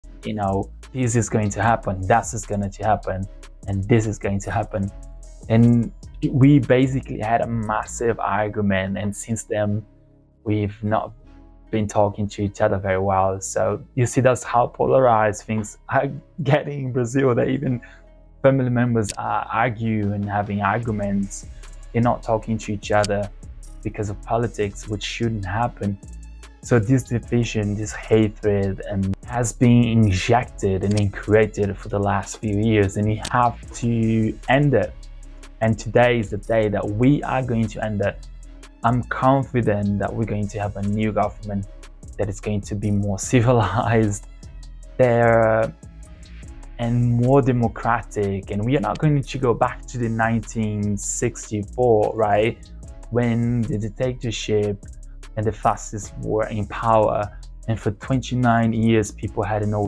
Brazilian General Election | 2.10.2022 | P3
From 1964 to 1989, with the military dictatorship in place, Brazilians did not have democratic means to cast their vote.
Since that troubled period of Brazil's history, there have been eight post democratic presidential elections.